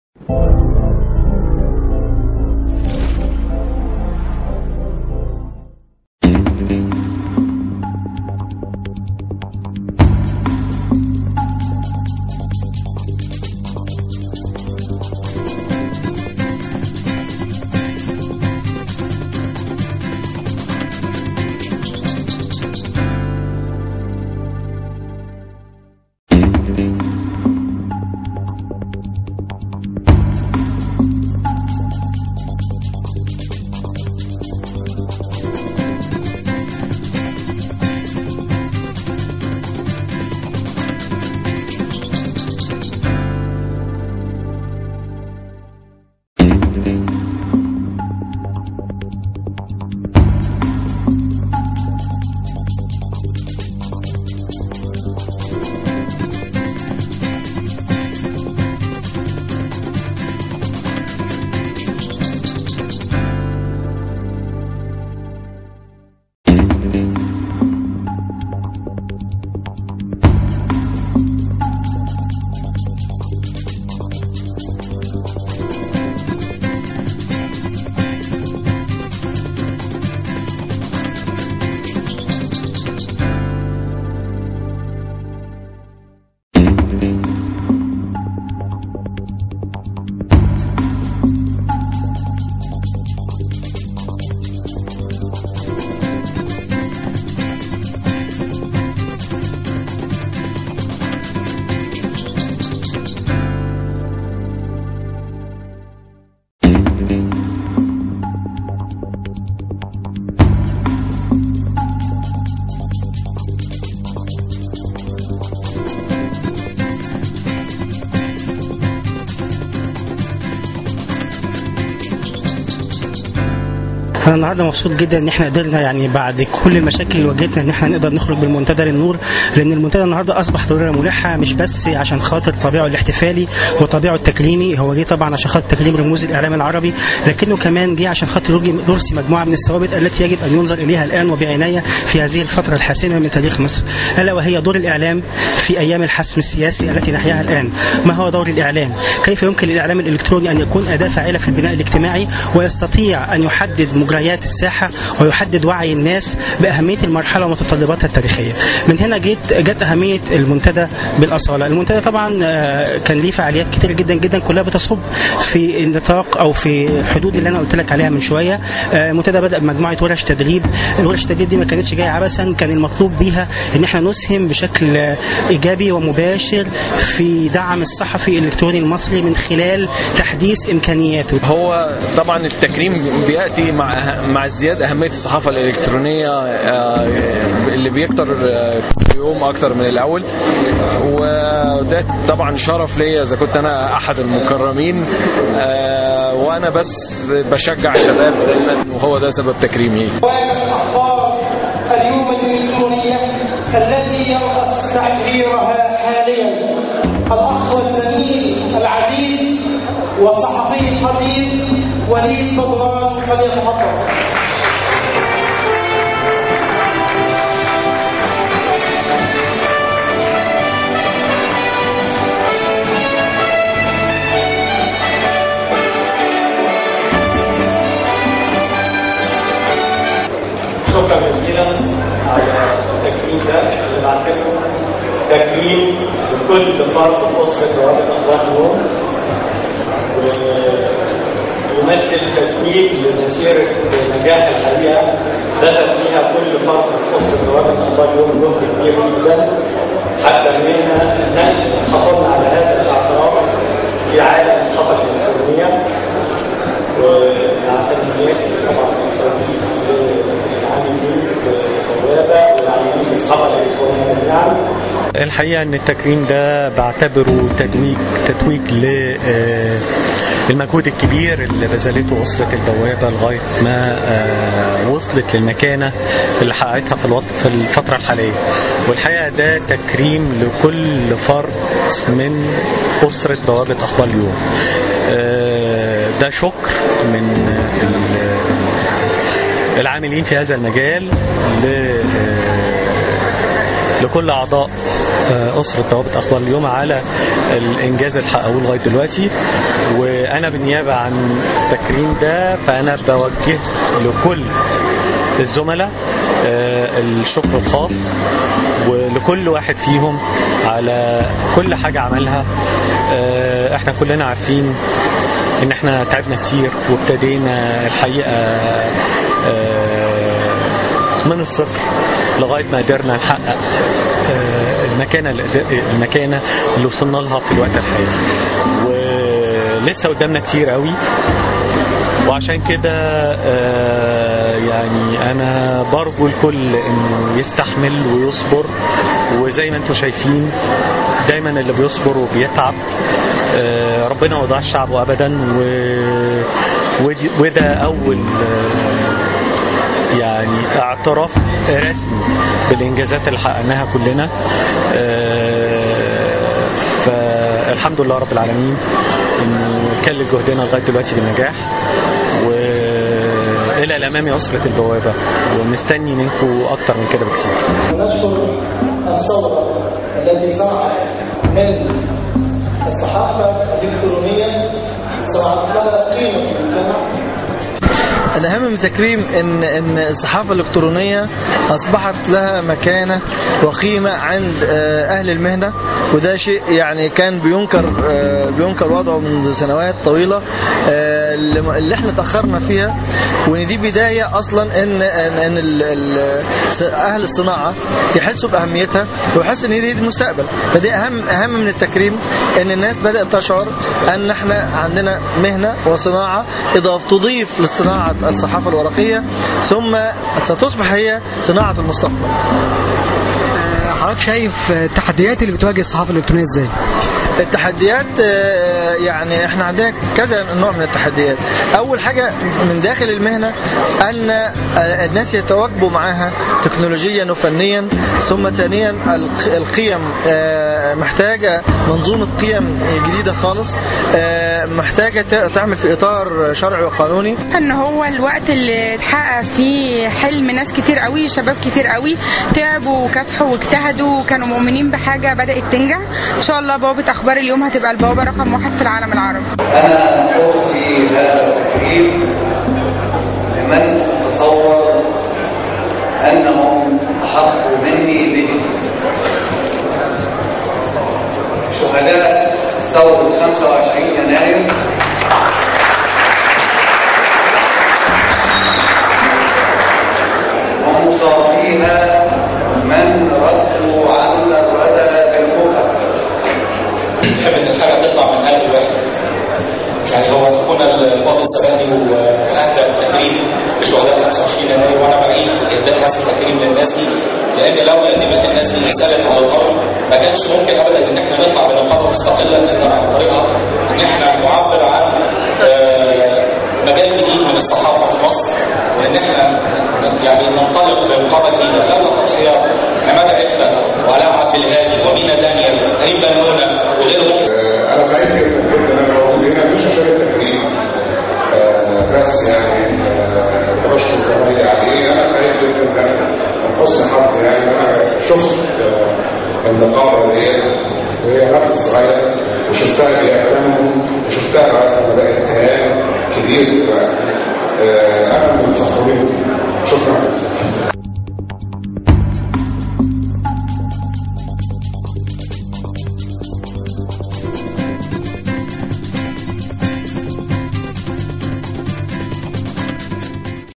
تكريم بوابة أخبار اليوم فى منتدى الصحافة الإلكترونية
تكريم الصحفى القدير الأستاذ / وليـــد بـدران ،رئيس تحرير بوابة أخبار اليوم كأحد رواد وصناع الصحافة الإلكترونية المتميزة بمصر فى الحفل الختامى لفعاليات منتدى الصحافة الإلكترونية الثانى الذى تنظمه نقابة الصحفيين الإلكترونيين .